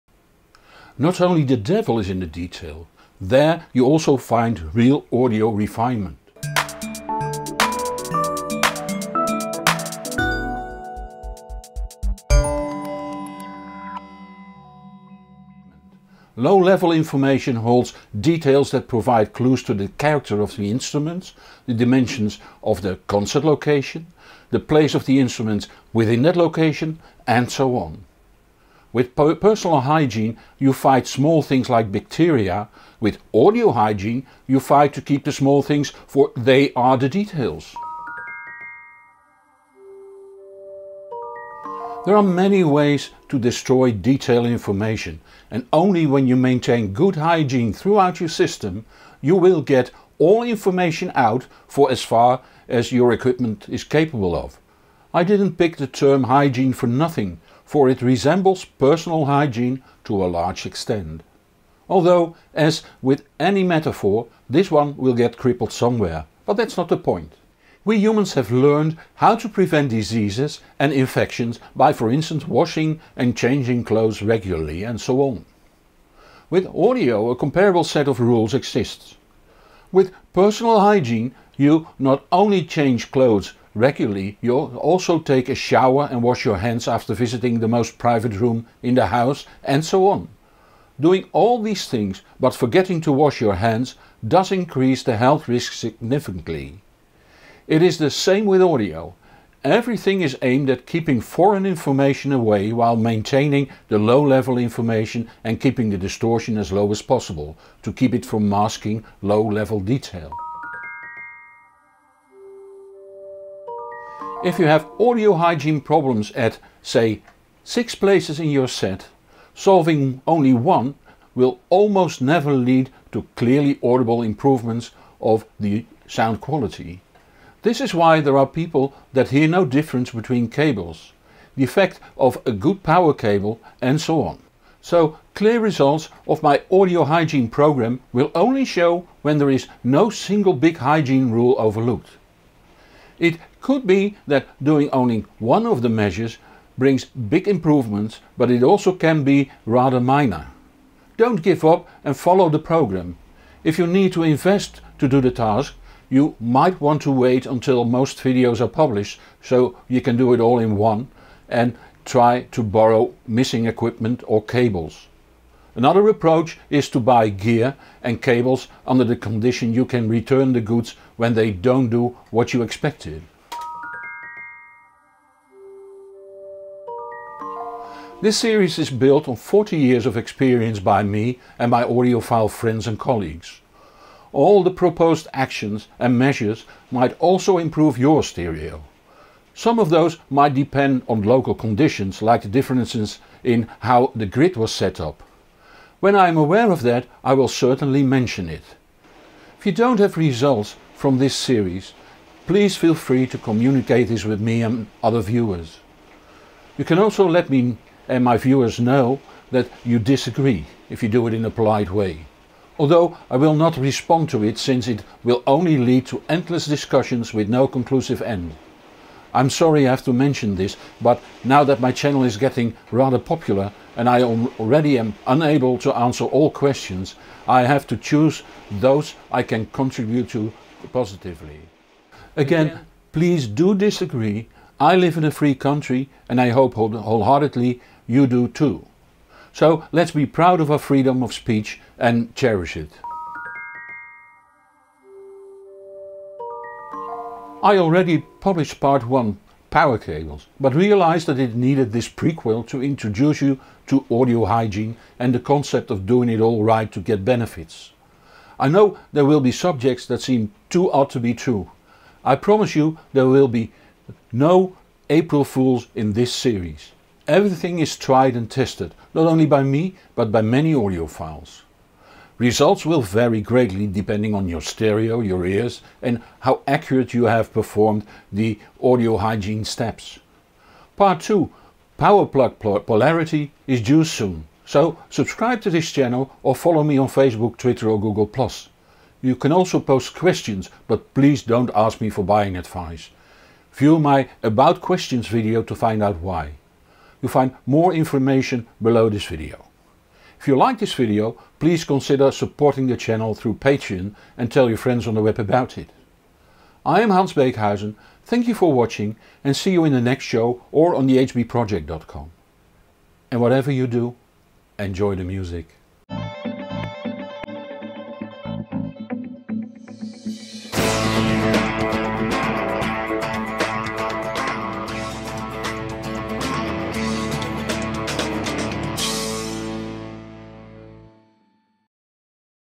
Audio Hygiene part 0: what about it
Not only the devil is in the detail, there you also find real audio refinement. Low level information holds the details that provide clues to the character of the instruments, the dimensions of the concert location, the place of the instruments within that location and so on. With personal hygiene you fight small things like bacteria, with audio hygiene you fight to keep the small things for they the details.
(English and Dutch subtitles - Nederlands ondertiteld)

English written edition: TBA
Nederlandse geschreven versie: NNB